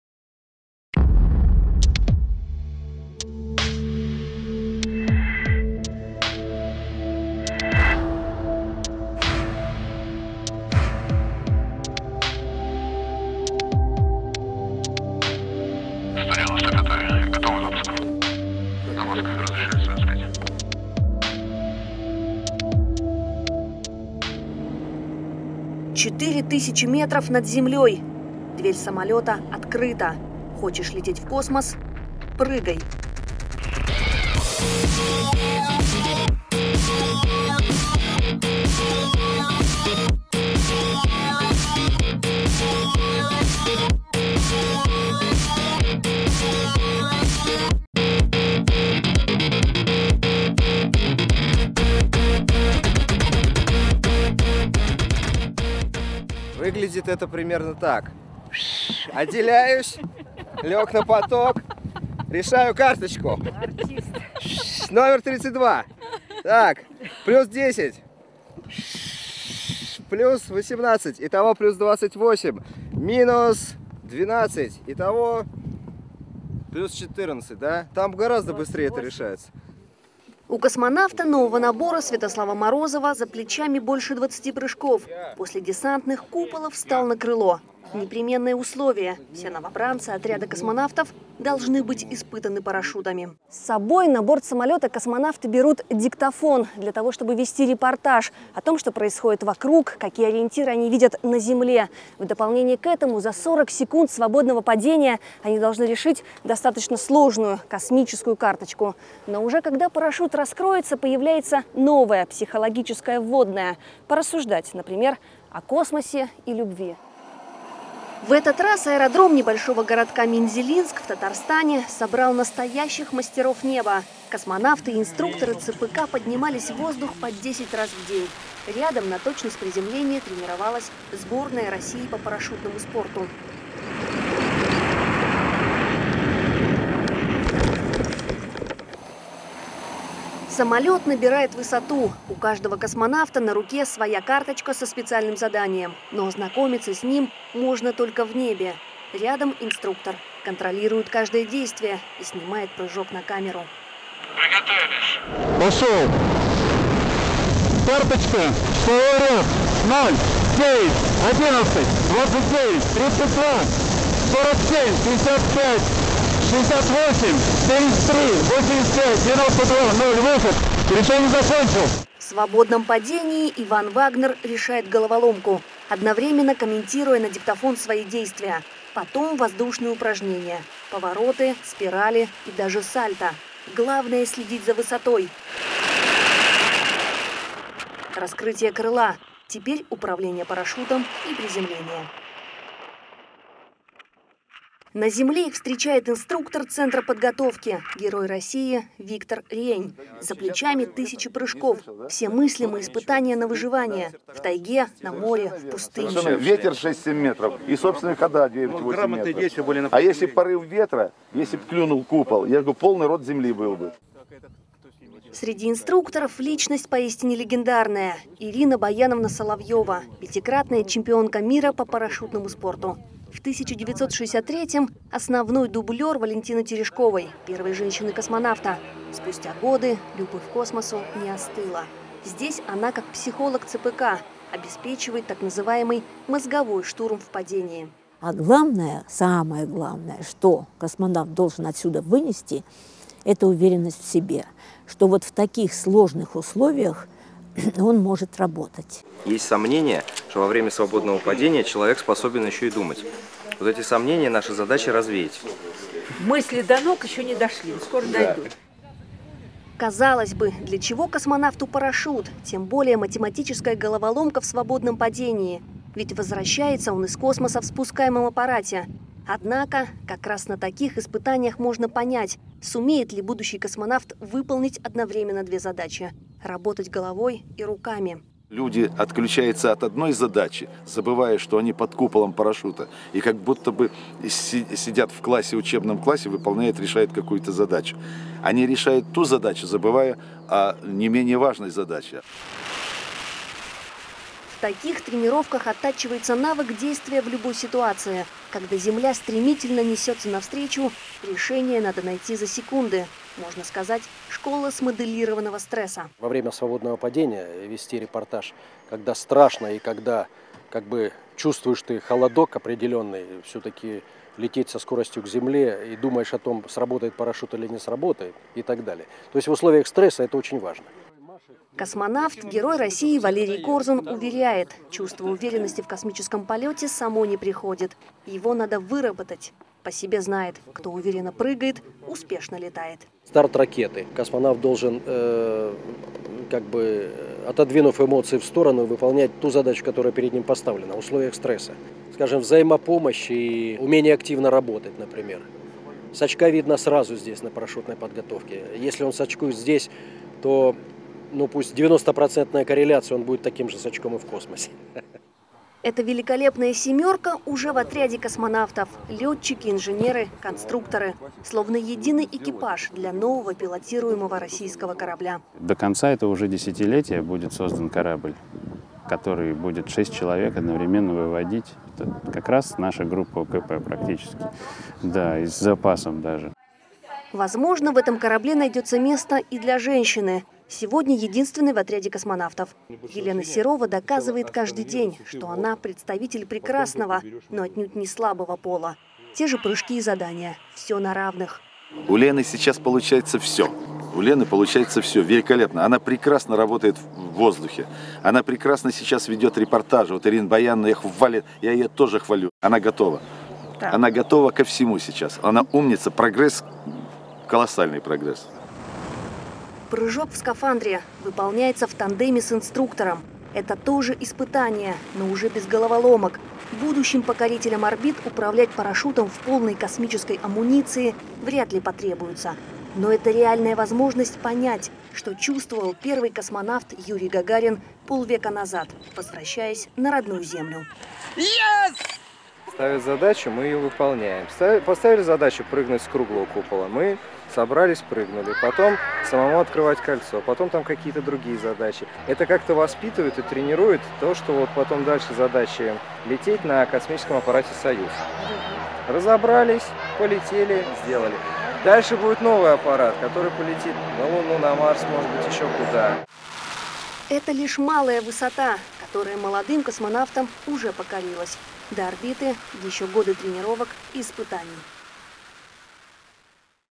Прыжок космонавта.
С 11 июня 2011 года, началась специальная парашютная подготовка космонавтов. Тренировки проходят в г. Мензелинск (республика Татарстан). В ходе трёхнедельной подготовки космонавты будут выполнять прыжки с парашютом, действуя в экстремальных условиях. В программу входят, помимо выполнения парашютных прыжков, решение задач информационного поиска, ведение репортажа во время свободного падения и другие упражнения. Тренировка продлится до 3 июля.